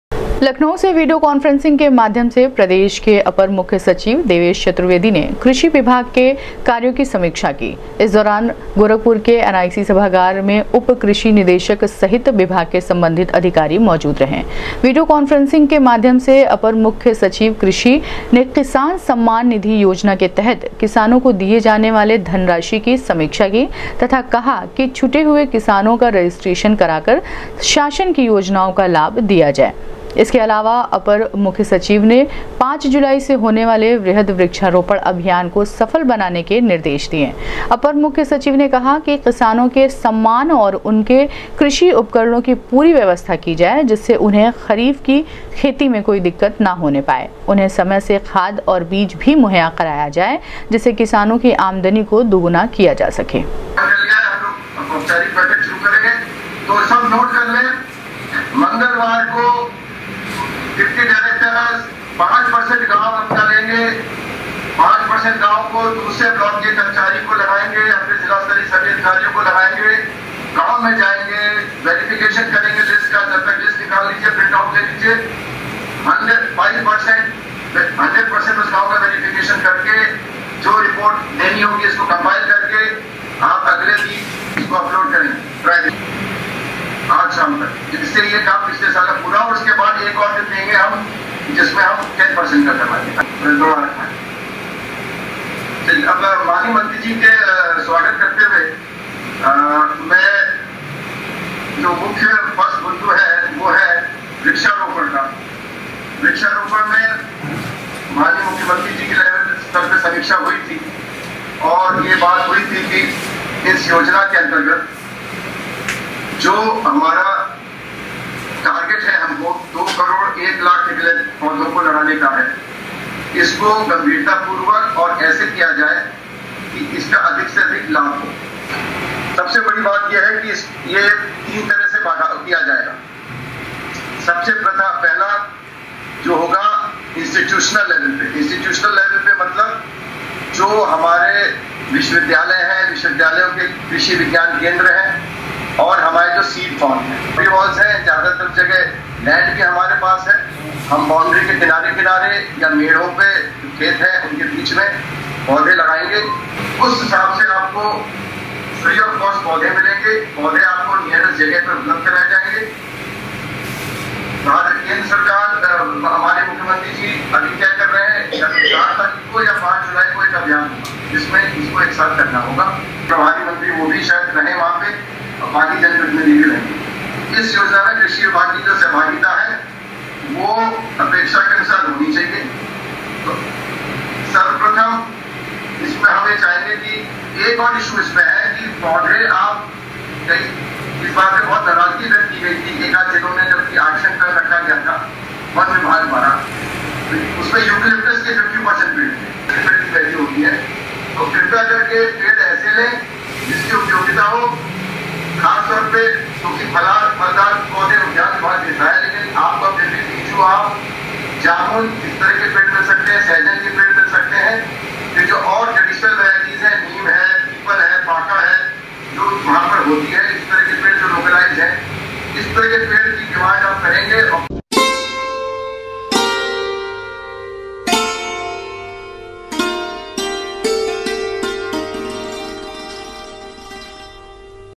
लखनऊ से वीडियो कांफ्रेंसिंग के माध्यम से प्रदेश के अपर मुख्य सचिव देवेश चतुर्वेदी ने कृषि विभाग के
लखनऊ से वीडियो कांफ्रेंसिंग के माध्यम से प्रदेश के अपर मुख्य सचिव देवेश चतुर्वेदी ने कृषि विभाग के कार्यों की,की समीक्षा, दिए आवश्यक दिशा निर्देश